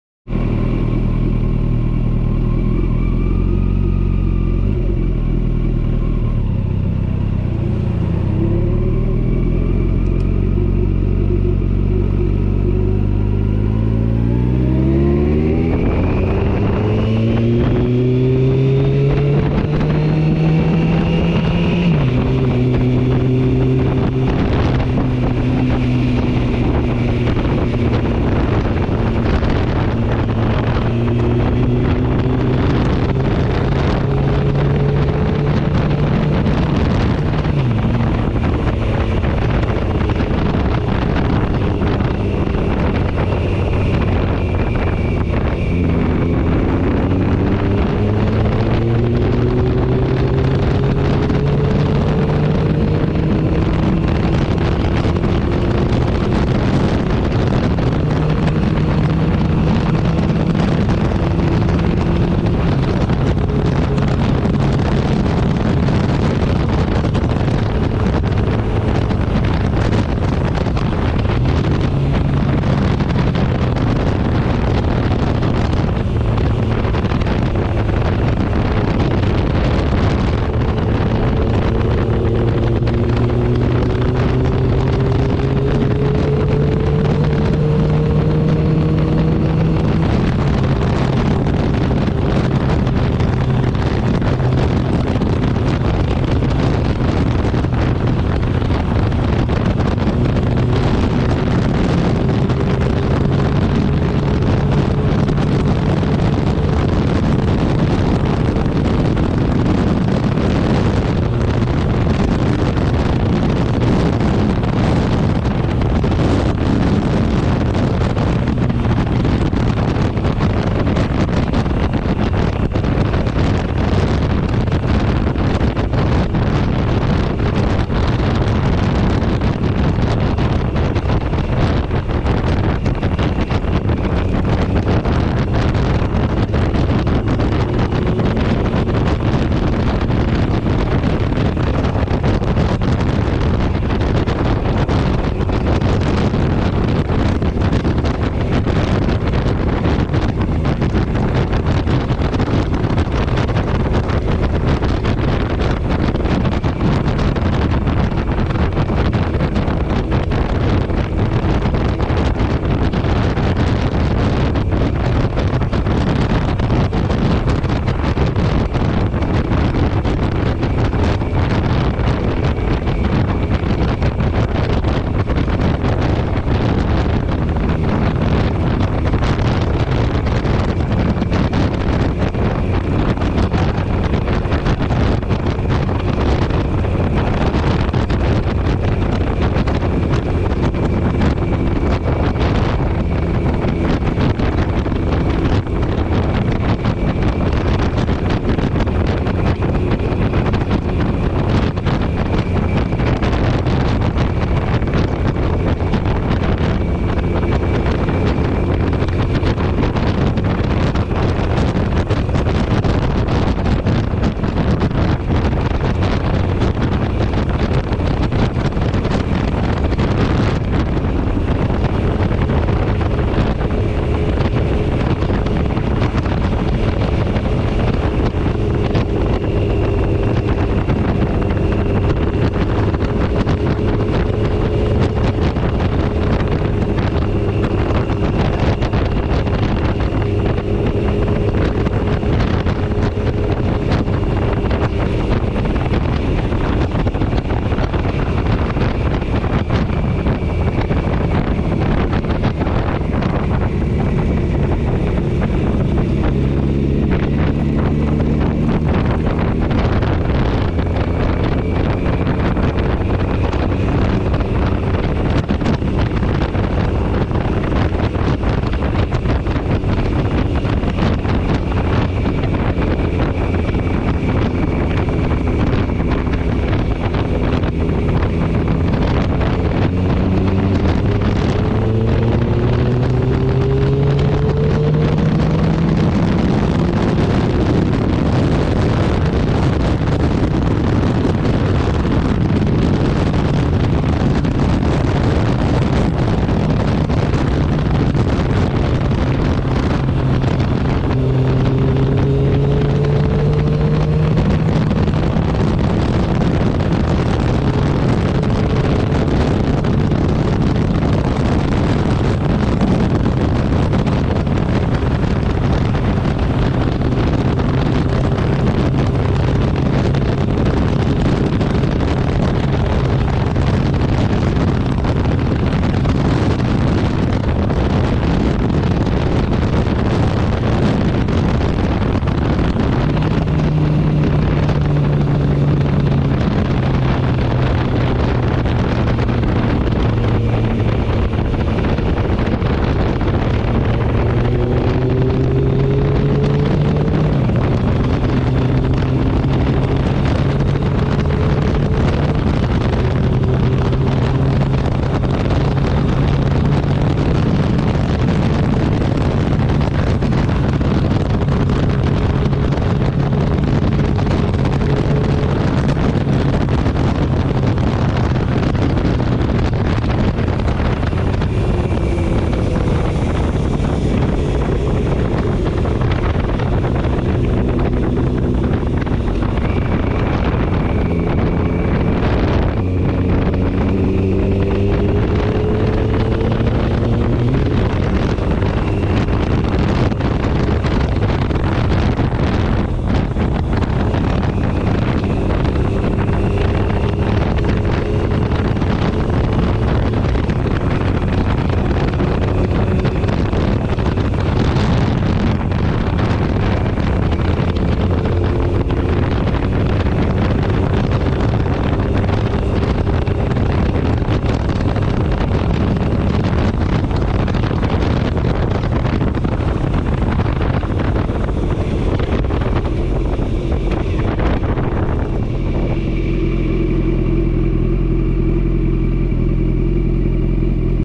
Lebanon Ohio Stubbs Mills to Morrow Ohio Road 22
Video is Purposely Raw as in "no editing"...Why?   As a watcher videos I understand the need to make edits, but I want to see the whole videos "just because" I like to ride.  So I am willing to watch a video like this one warts and all.